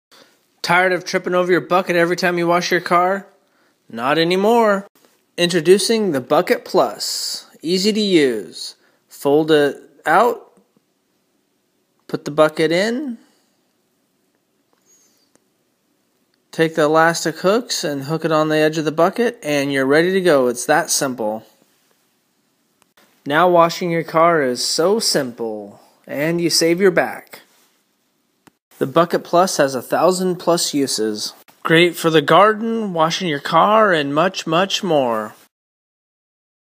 Bucket Plus, best bucket holder in the world.
The Bucket Plus is handy for washing you car, painting, gardening,  pitching batting practice and much, much more.